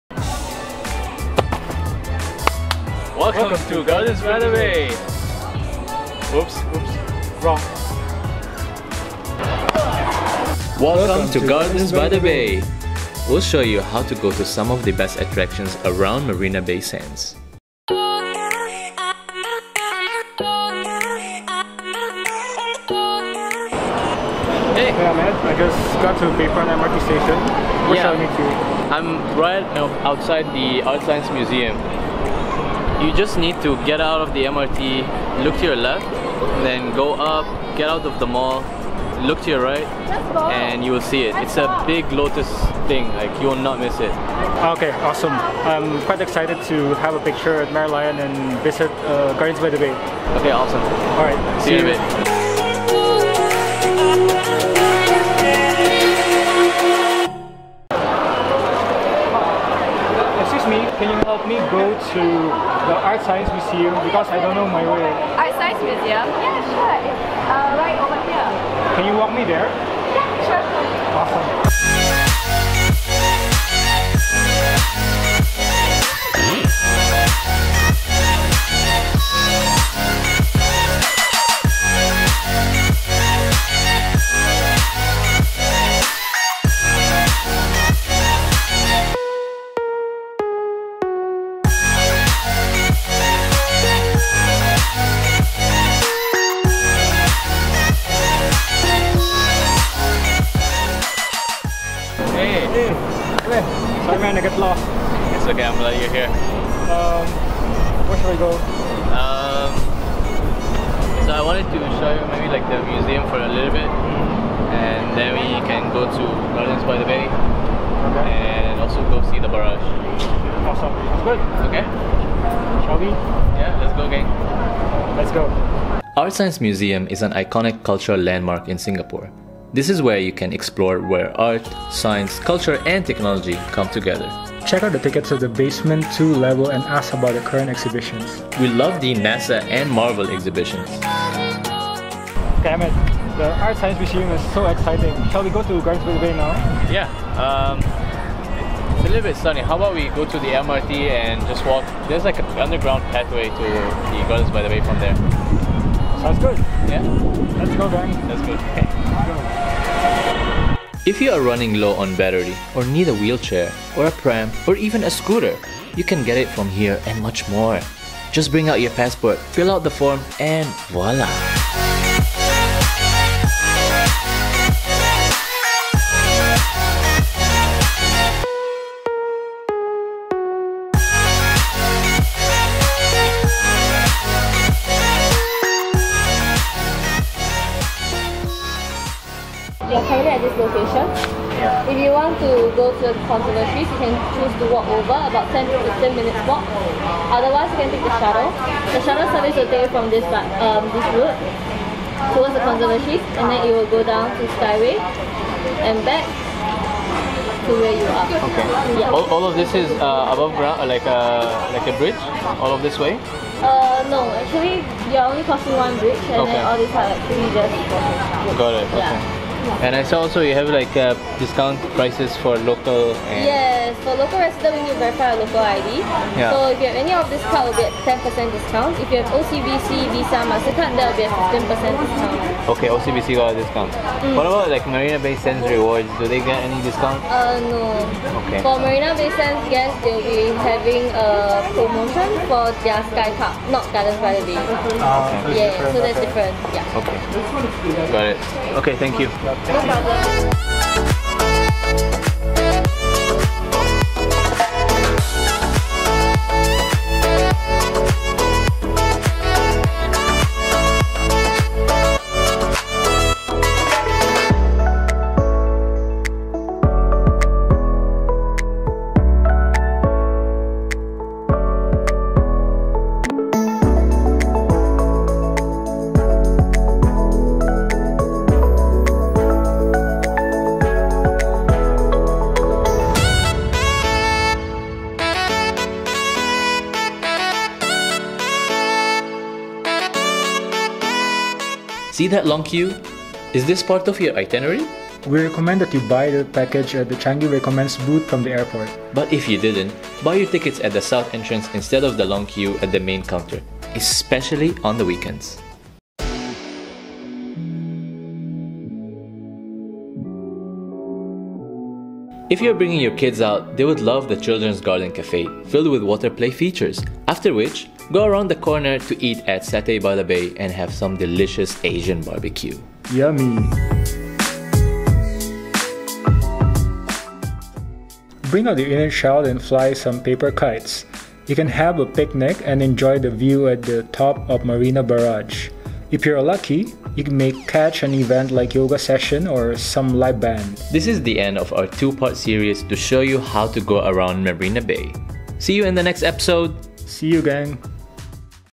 Episode 2: Marina Bay Attraction & Tips When You Visit Gardens by the Bay. Part 1/2
We are here to show you around Marina Bay Sands and visit popular locations like Art Science Museum, Gardens by the Bay and all the way to Marina Barrage. Enjoy!

◈✔️ Equipment we use for filming ✔️◈